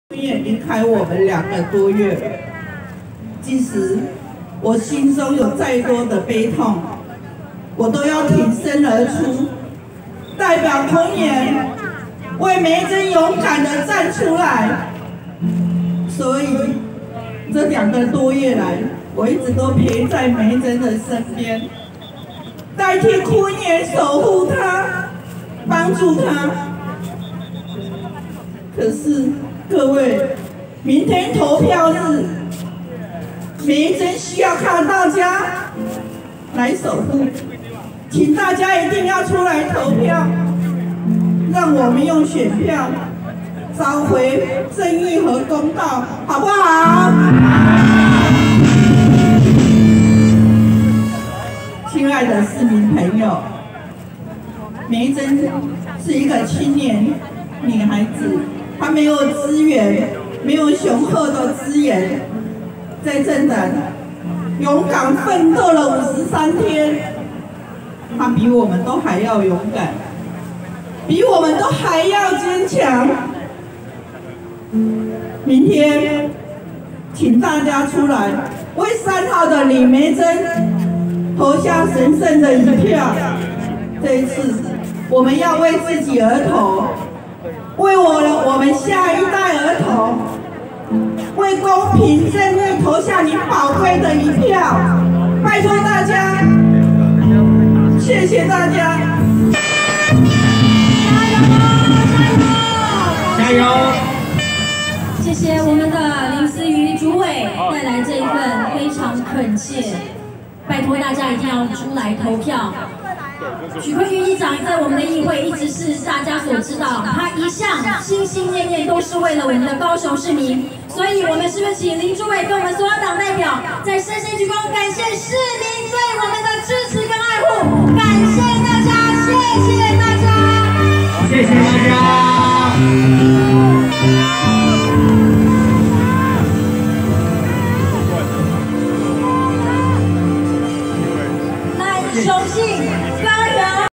亞洲全球新聞（記者陳文娟，胡志忠/高雄報導）2
李眉蓁高雄市長補選選前之夜在夢時代，議長夫人出來站台。